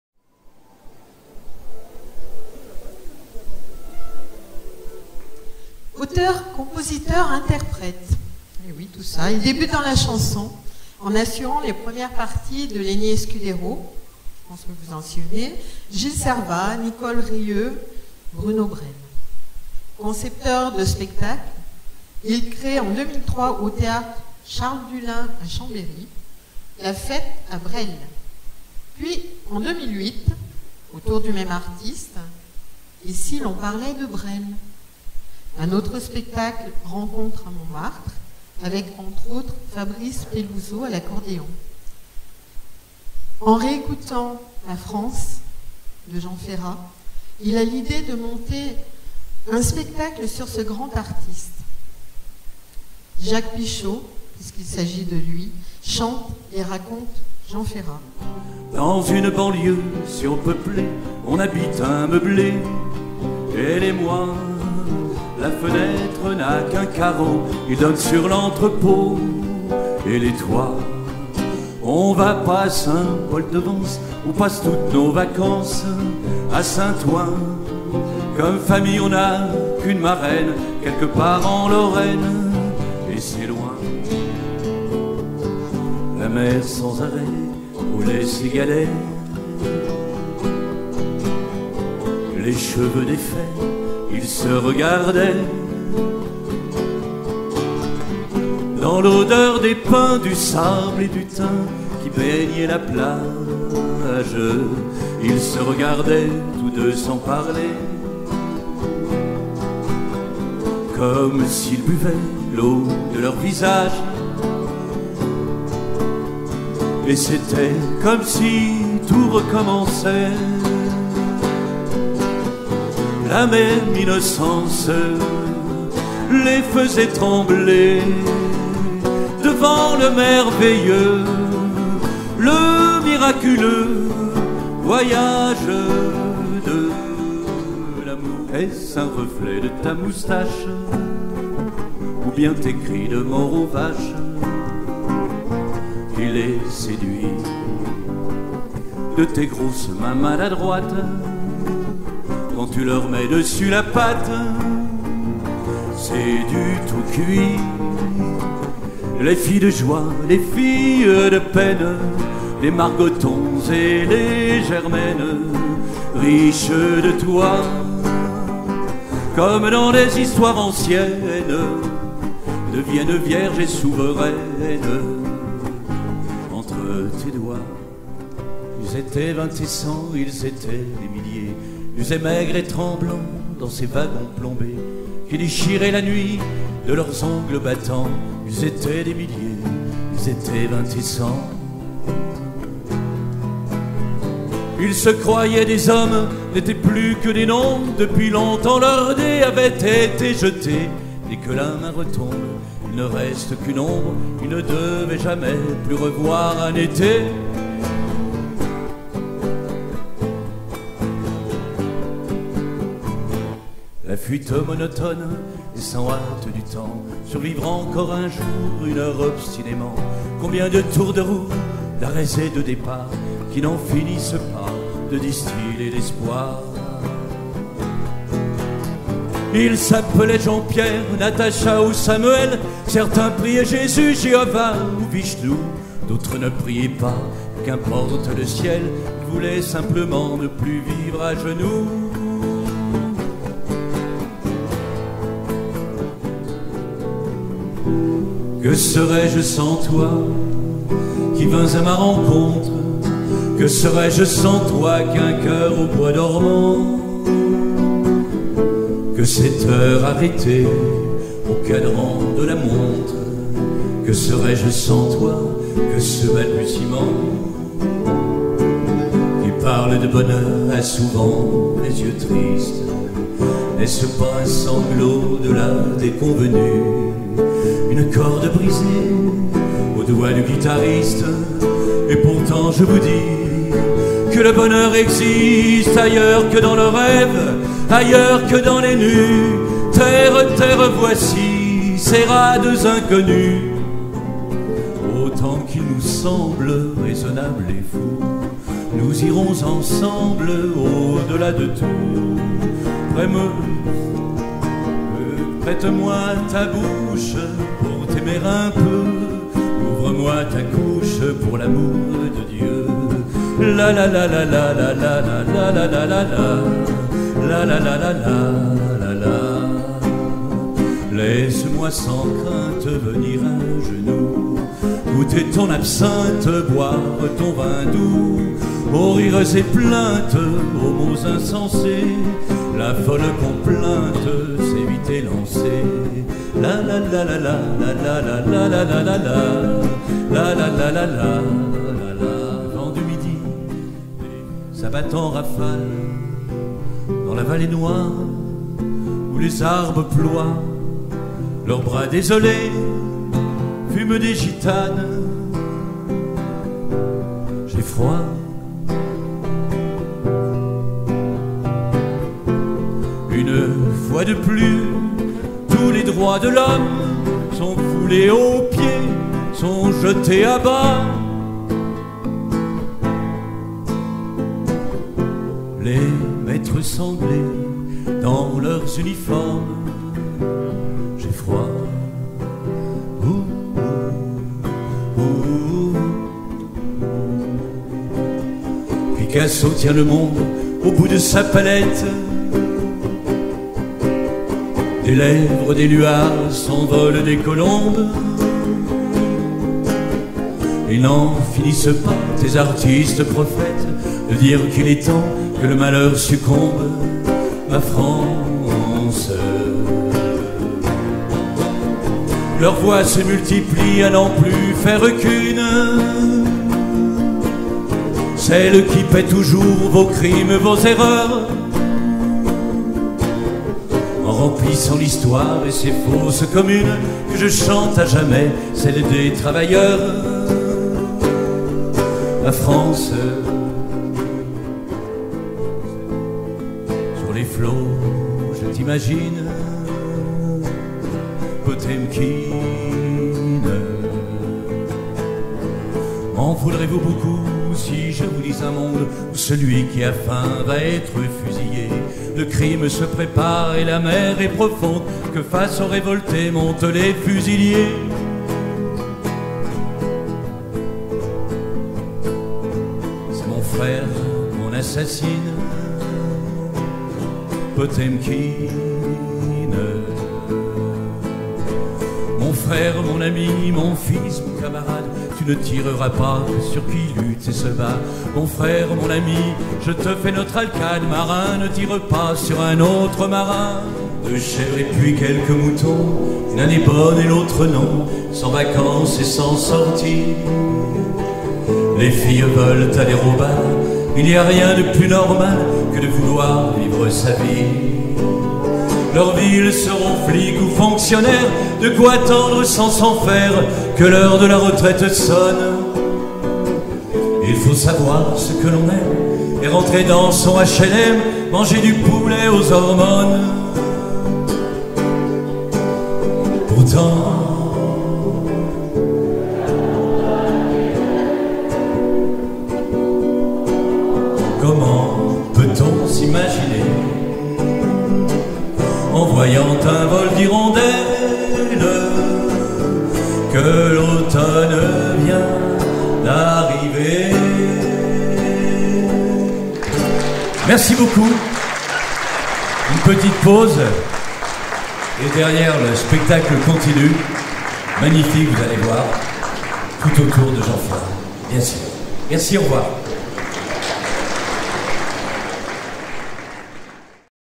Jacques Pichaud chante Jean Ferrat
Chansons de J. Ferrat chantées par Jacques Pichaud le dimanche 03 mars 2013 lors de la soirée organisée par l'association "Vivre à Chirens" (38).